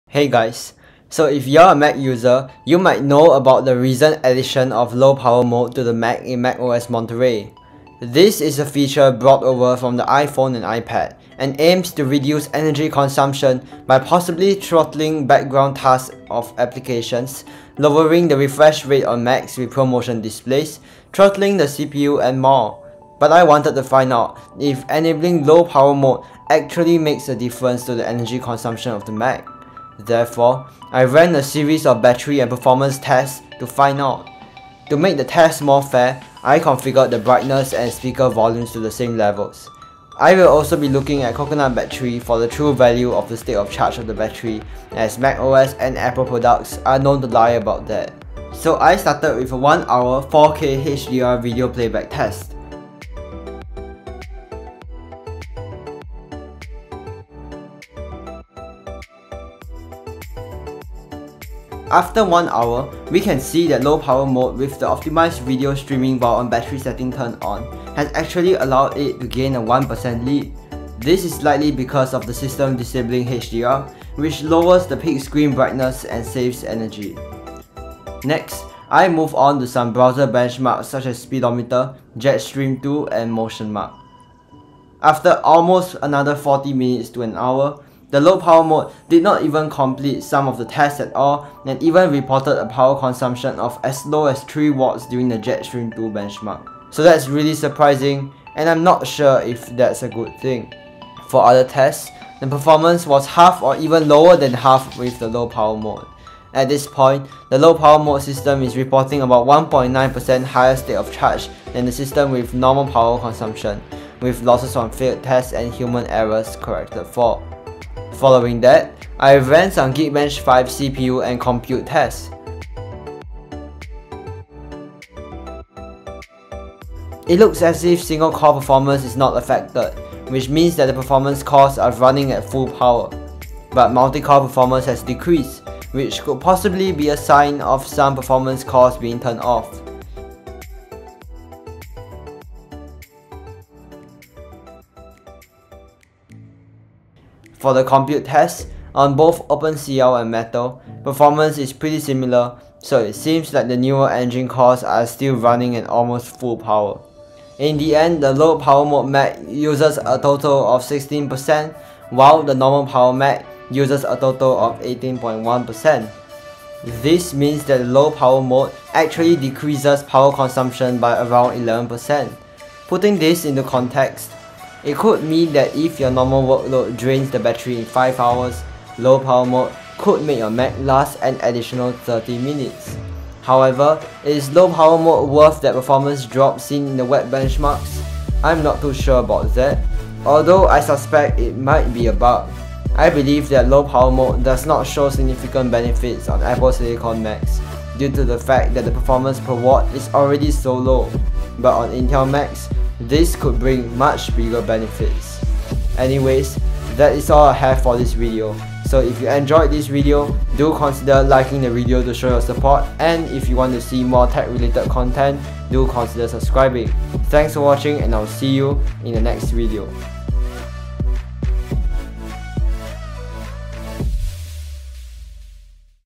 Does Low Power Mode actually improve an Apple Silicon Mac's battery life?
In this video, I run some battery tests to find out if Low Power Mode on macOS is actually worth using.

0:00 Introduction
0:48 Video playback test
1:18 Browser benchmark test
1:58 Geekbench 5 test
2:38 Conclusion